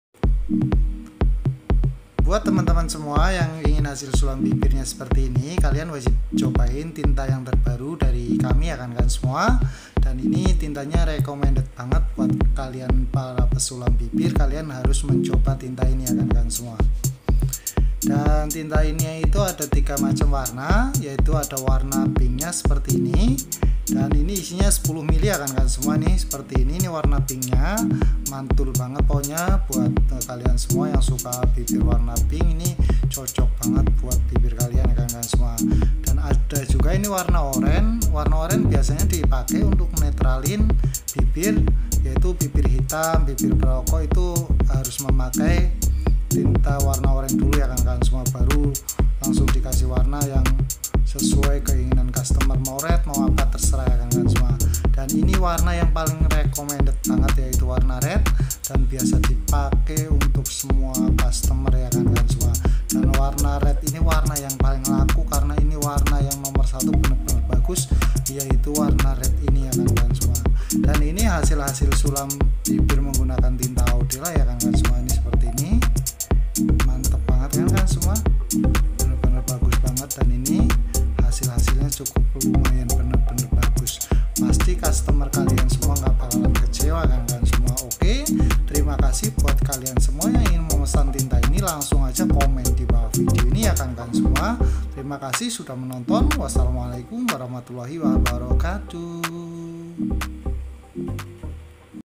Recomended tinta sulam bibir yang bagus|| ink lips Audela
buat kalian yang masih ragu buat pemilihan tinta bibir yg bagus ,kalian harus pake ini ,tinta audela hasil sulam bibir jadi mantul banget

buruan yg mau langsung chat admin ya

l